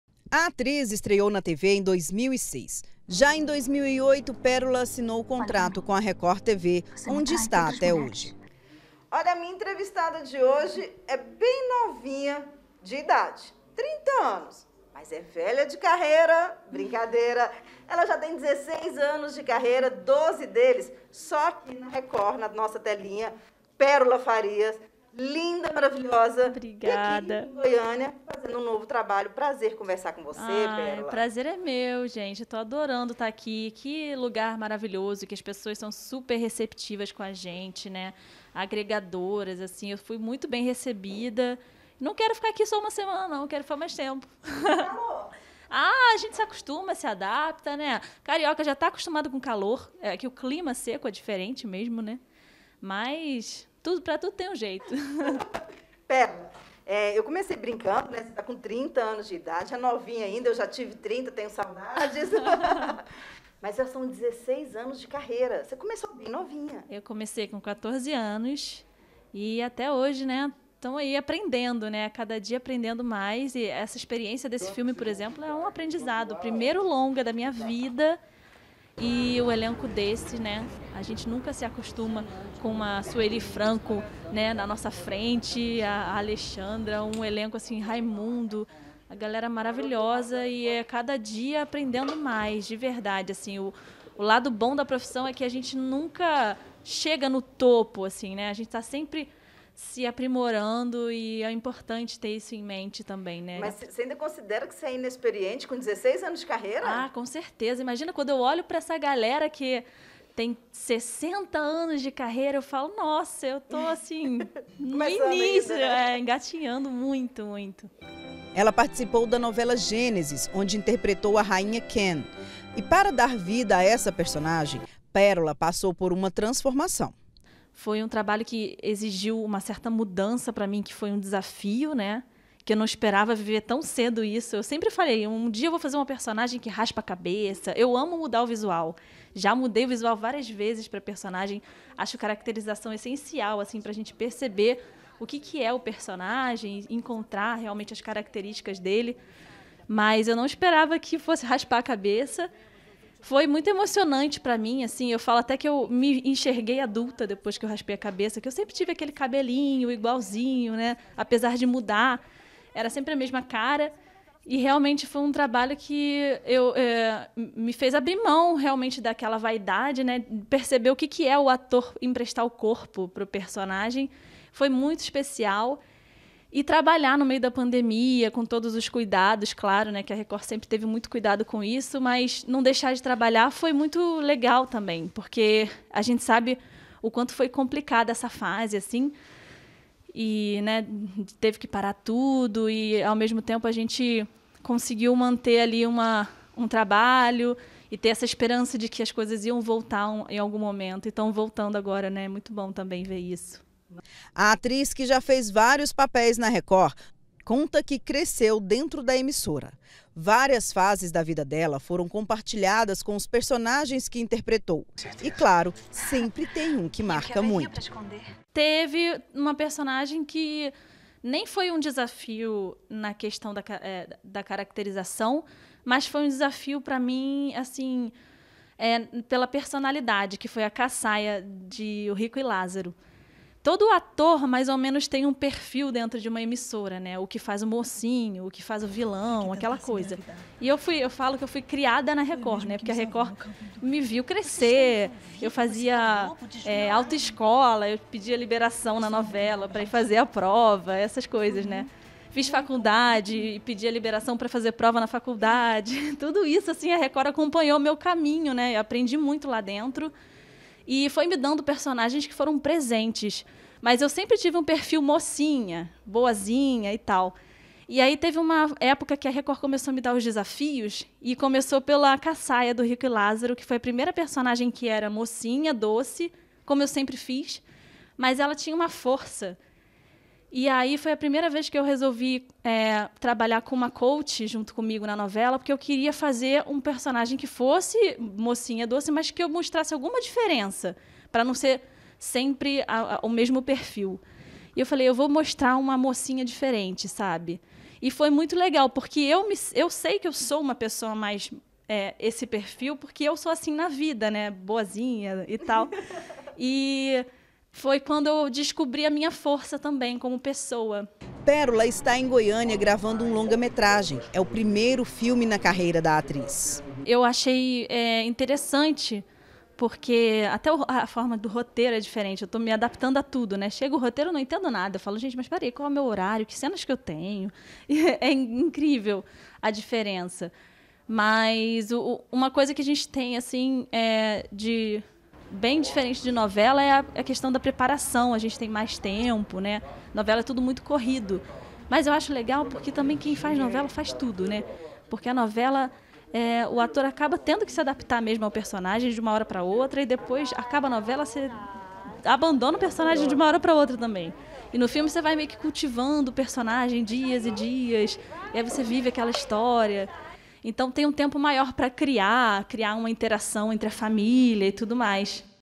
HORIZONTE: ATRIZ PÉROLA FARIA GRAVA O PRIMEIRO FILME DA CARREIRA EM GOIÁS
O ATRIZ PÉROLA FARIA ESTÁ EM APARECIDA DE GOIÂNIA PARA GRAVAR O PRIMEIRO FILME DA CARREIRA. EM UM BATE-PAPO COM A CRISTAL, A ATRIZ CONTA SOBRE OS 16 ANOS DE CARREIRA.